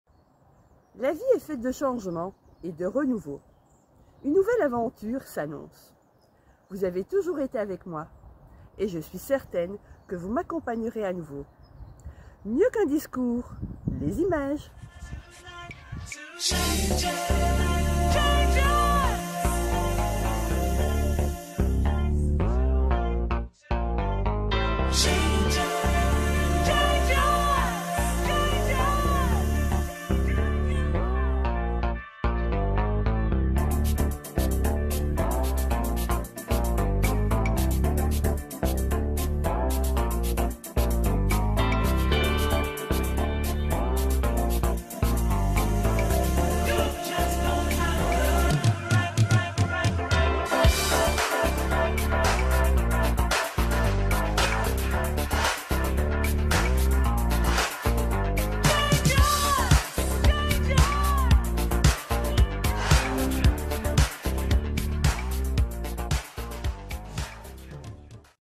Lingerie Maggy vous présente son nouveau partenaire lingerie !!
La vie change, l'esprit reste... Je reste toujours dans l'optique de vous assurer le maximum de confort, de raffinement et de fiabilité.

J'ai l'immense plaisir de vous présenter mon nouveau partenaire "lingerie", ALLANDE. Du made in France, c'est joli, raffiné et confortable, quoi de mieux ?

Je suis certaine que vous m'accompagnerez dans cette nouvelle aventure, et que j'aurai toujours l'immense plaisir de vous retrouver.

A tout bientôt !